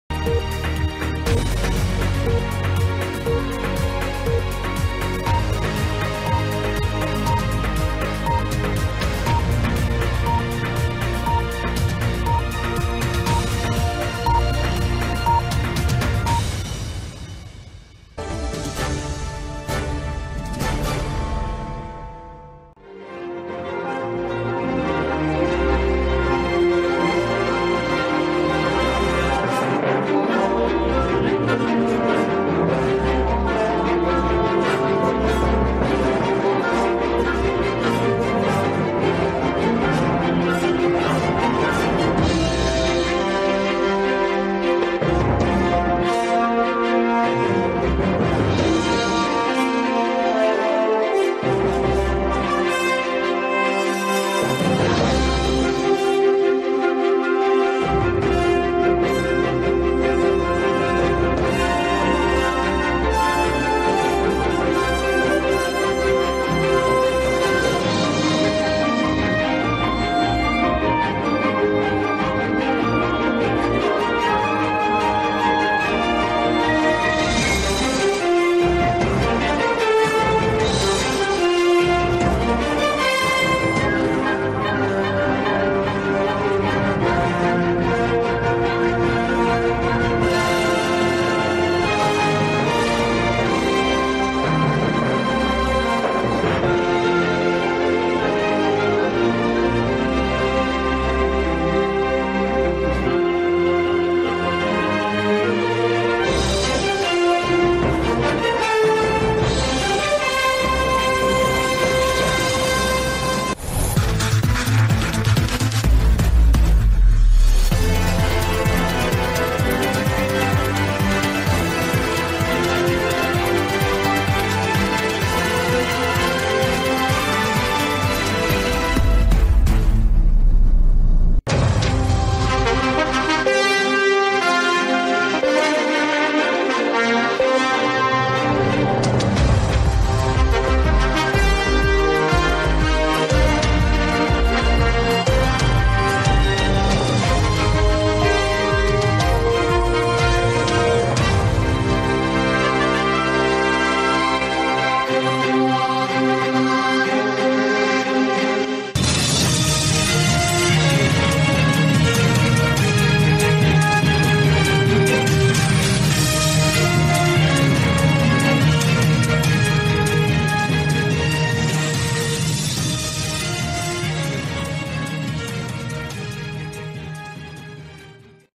България | Парламентарни Избори ЗАКЛЮЧИТЕЛНА Прогноз 2023 БОPИСОВ VS ПЕТКОВ ЕКСКЛУЗИВНИ ДАННИ
Здравейте на всички, това са Globe Elections UN.
Добре дошли в новото ни видео - 2023 ЗАКЛЮЧИТЕЛНА Прожекция / Предвиждане / Прогнозза Парламентарни Избори в България „БОПИСОВ VS ПЕТКОВ“.
Абонирайте се, харесвайте, коментирайте и споделяйте, особено ако се интересувате от избори!!!
Благодаря ви и се наслаждавайте!!!